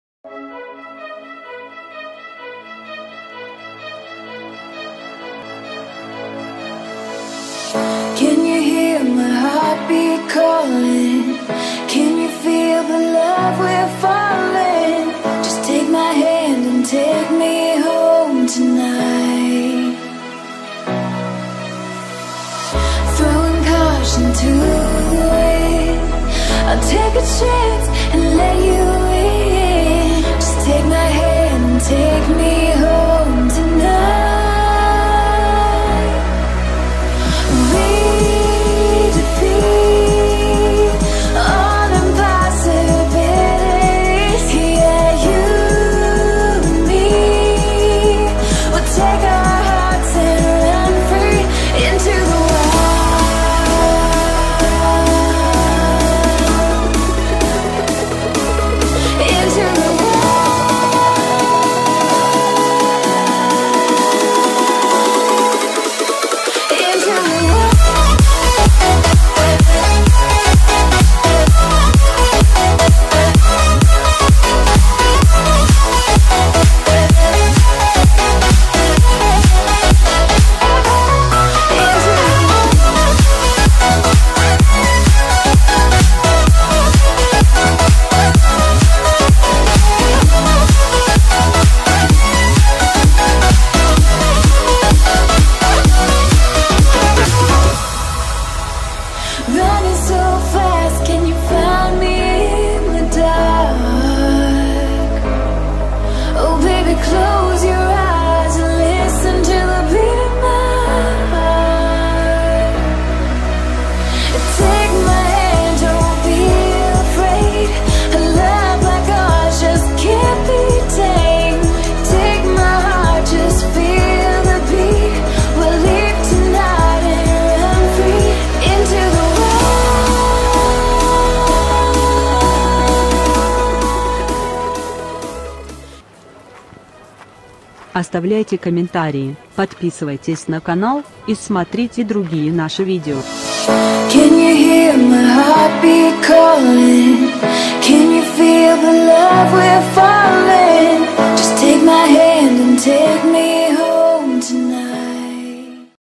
Белки отдыхают на пляжах Хорватии
Симпатичный белки и в купальниках и с хвостиками с огромным удовольствием отдыхают на красивых пляжах Хорватии